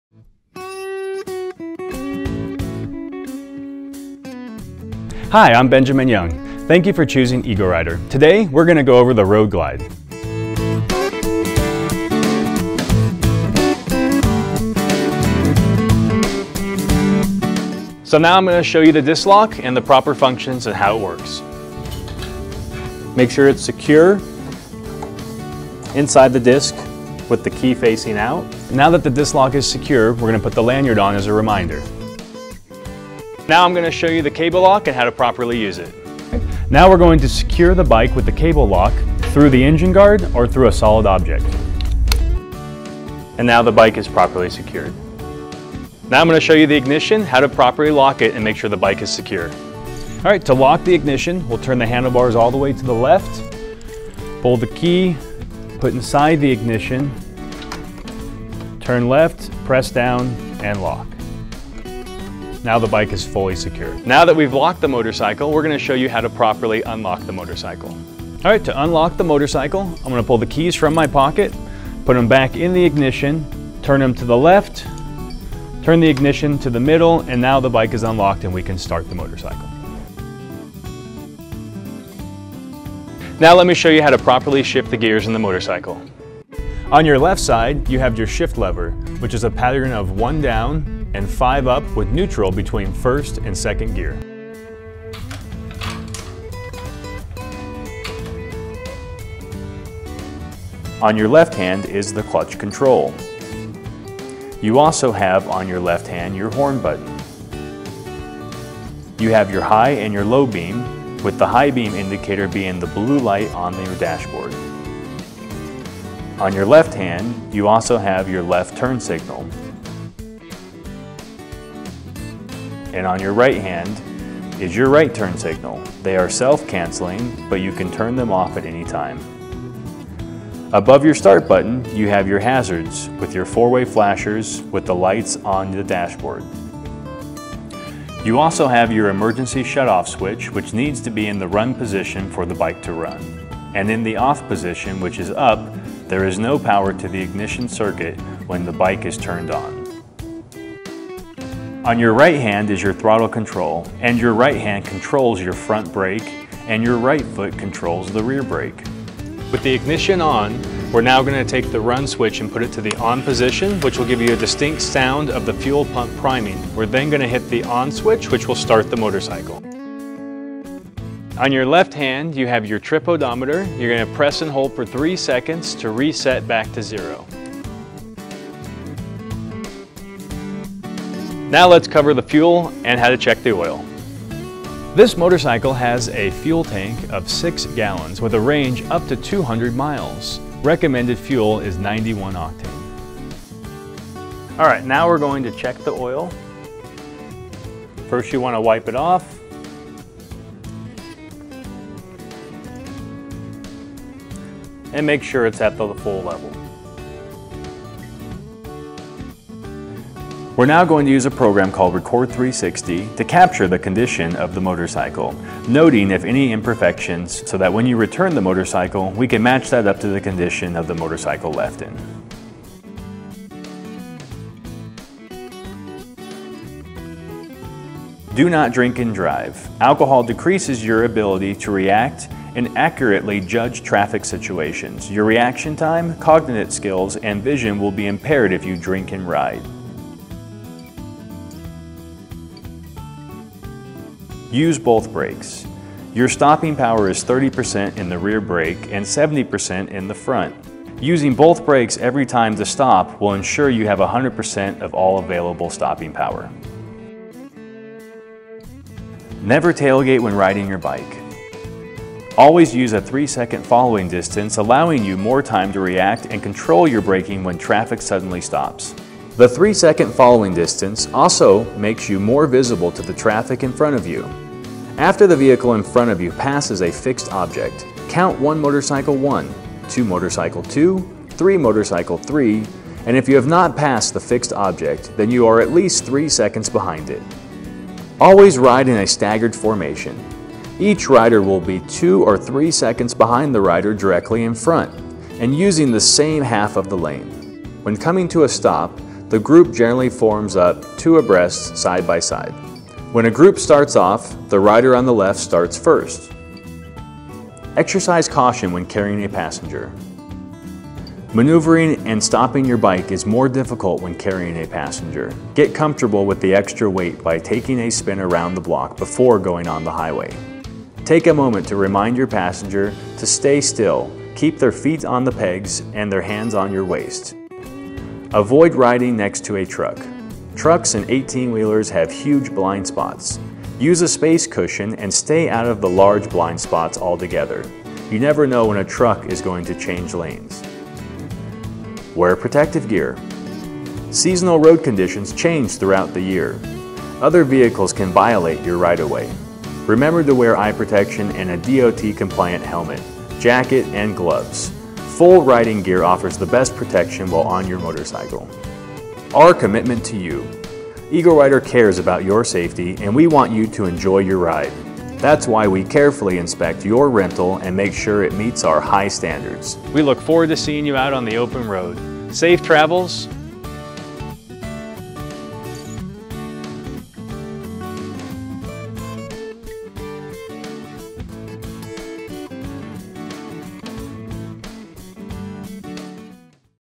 Harley-Davidson Road Glide Orientation | EagleRider
We're excited to get you on the road. Here's an overview of the Harley-Davidson Road Glide that you're renting from us and we've also included important tips for safe riding.